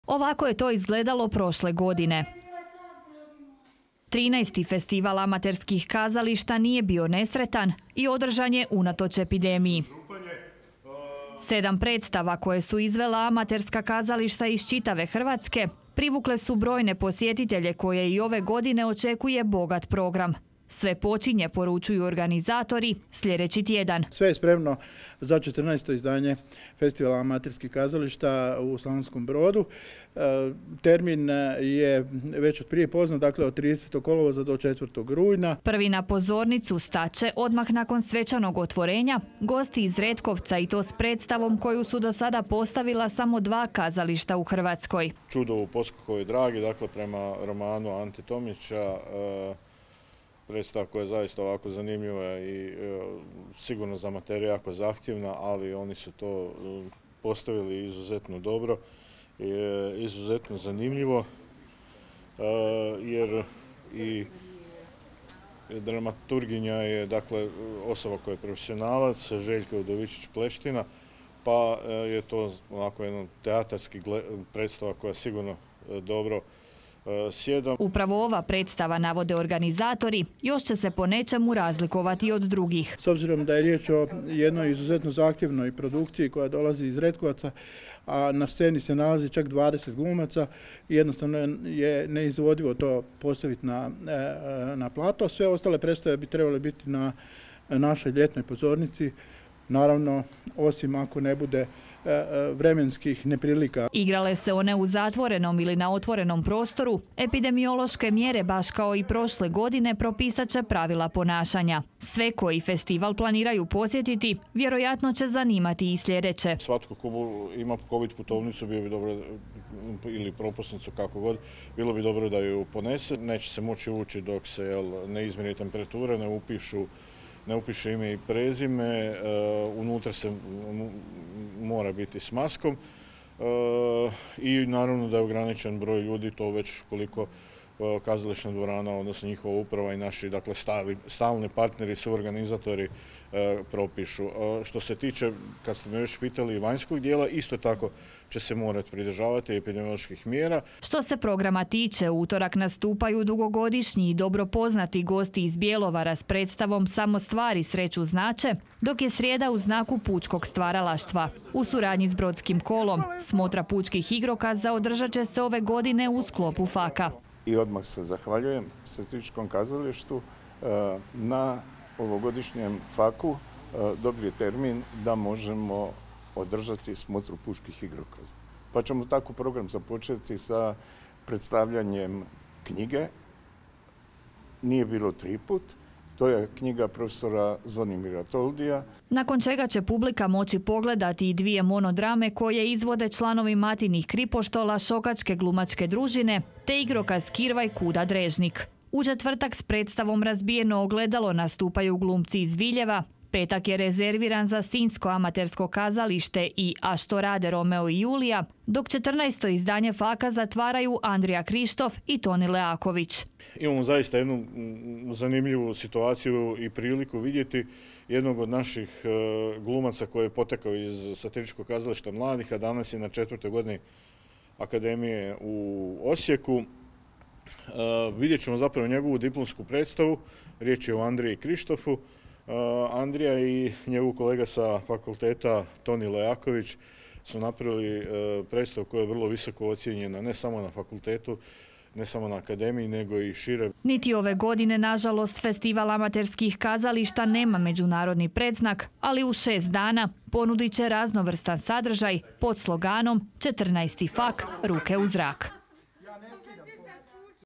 SBTV - DNEVNIK - NAJAVLJEN 14. FESTIVAL AMATERSKIH KAZALIŠTA - 25.08.2021.
Novo, 14. izdanje Festivala amaterskih kazališta u Slavonskom Brodu, počinje sljedećeg tjedna. I ove godine, Slavonski Brod bit će domaćin brojnih amatrskim kazalištima iz čitave Hrvatske, što posjetitelje očekuje, koji su noviteti, a što ostaje po starom, saznat ćete u nastavku.

DNEVNIK SBTV-a | PON-PET | 20:00 i 22:00